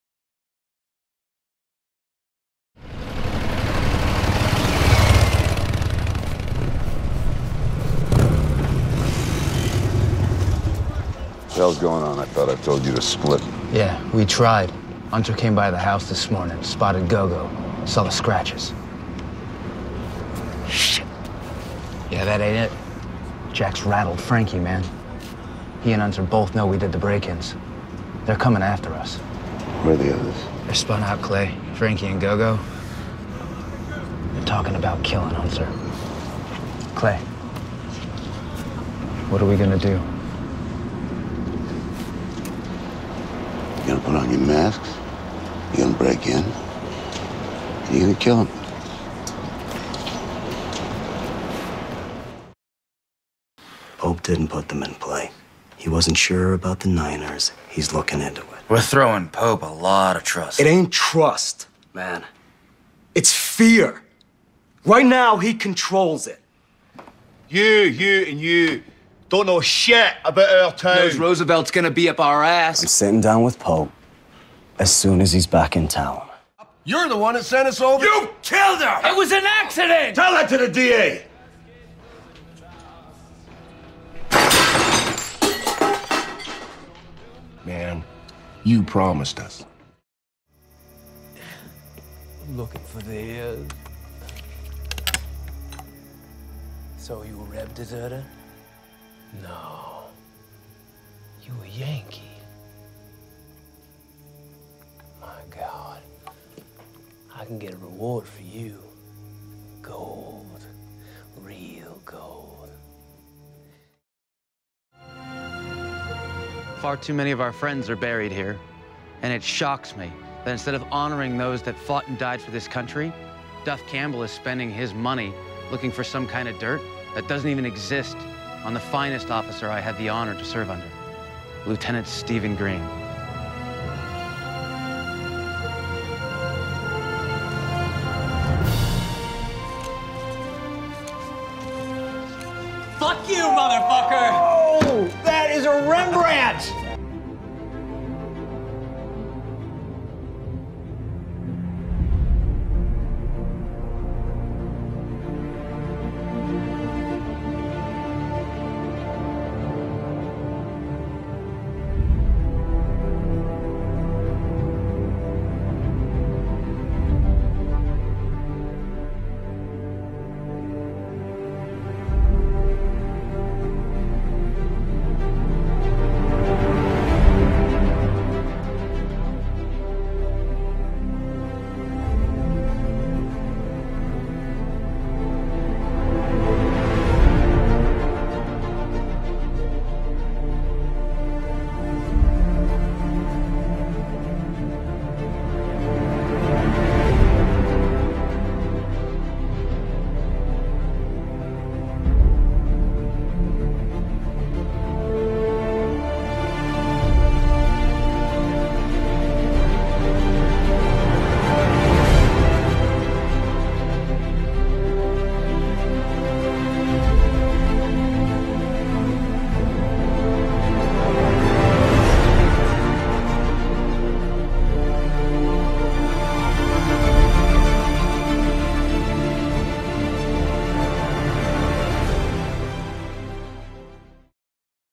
Kurt Yaeger - Reel
A short compilation of Kurt Yaeger's performances from film, tv and interviews.